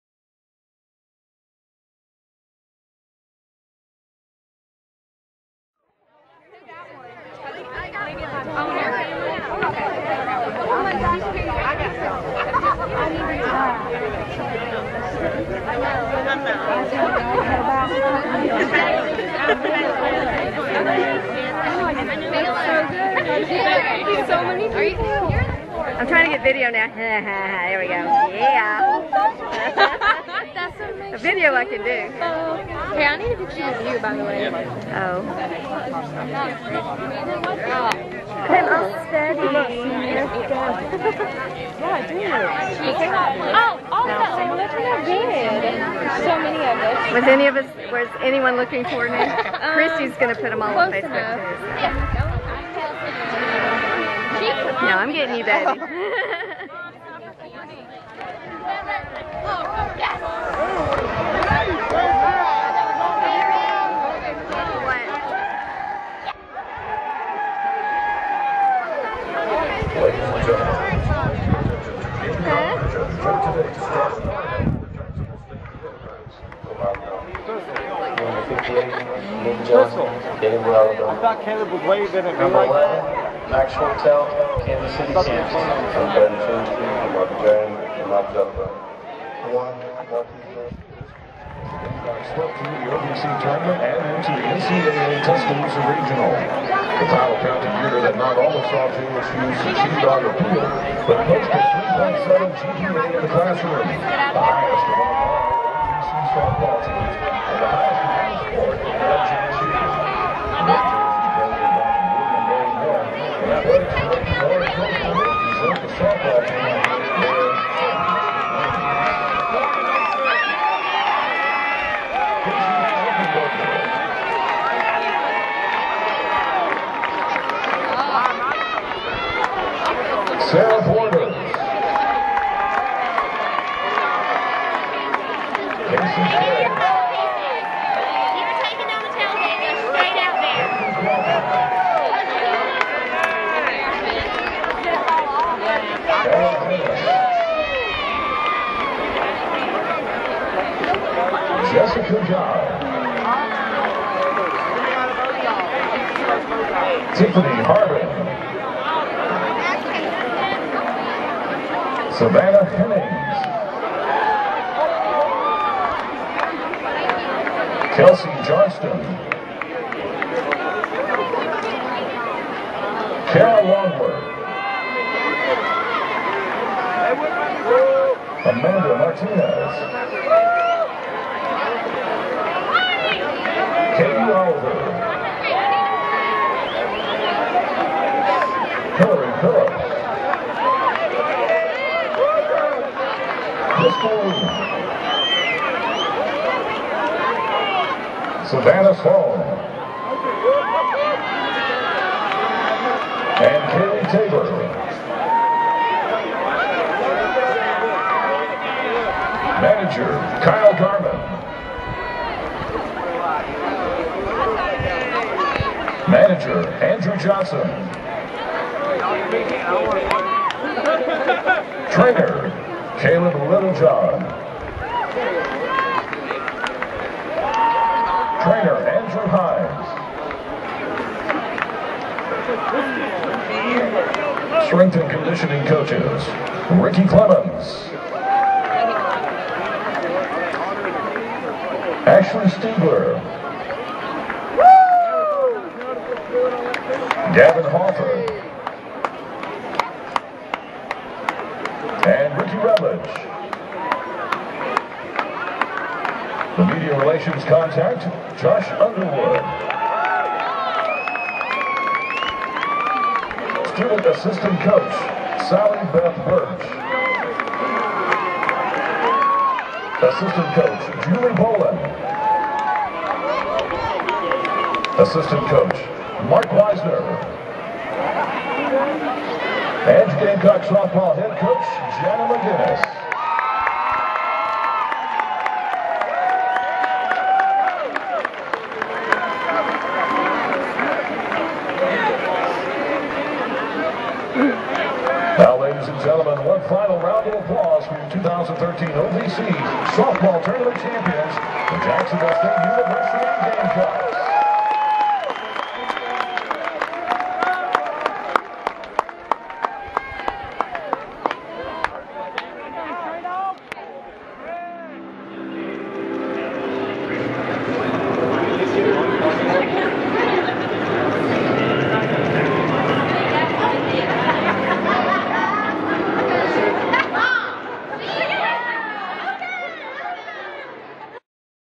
2013 Jacksonville State Softball OVC Championship Ring Ceremony
Sept. 7, 2013 ... prior to the JSU vs. Jacksonville University football game at Burgess Snow Field in Jacksonville, Ala.